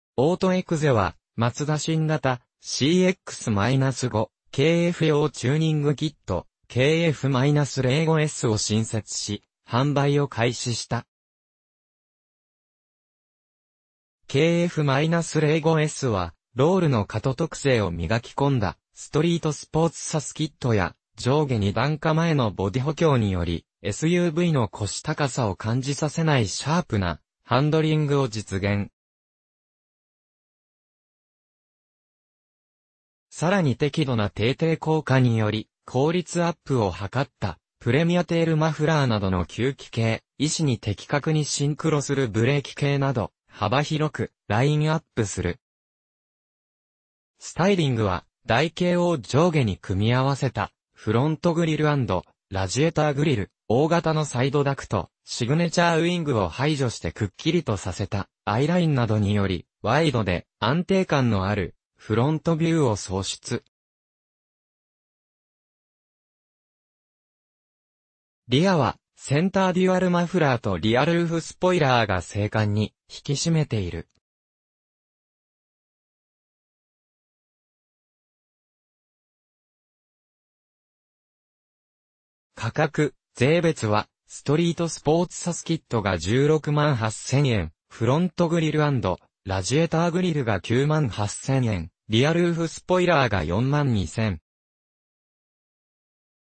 オートエクゼ、新型 CX-5 用チューニングキットを発売
オートエクゼは、マツダ新型『CX-5（KF）』用チューニングキット「KF-05S」を新設し、販売を開始した。KF-05Sは、ロールの過渡特性を磨き込んだストリートスポーツサスキットや上下2段構えのボディ補強により、SUVの腰高さを感じさせないシャープなハンドリングを実現。さらに適度な低抵抗化により効率アップを図ったプレミアテールマフラーなどの吸気系、意思に的確にシンクロするブレーキ系など、幅広くラインアップする。スタイリングは、台形を上下に組み合わせたフロントグリル＆ラジエターグリル、大型のサイドダクト、シグネチャーウイングを排除してくっきりとさせたアイラインなどにより、ワイドで安定感のあるフロントビューを創出。リアはセンターデュアルマフラーとリアルーフスポイラーが精悍に引き締めている。価格（税別）はストリートスポーツサスキットが16万8000円、フロントグリル＆ラジエターグリルが9万8000円、リアルーフスポイラーが4万2000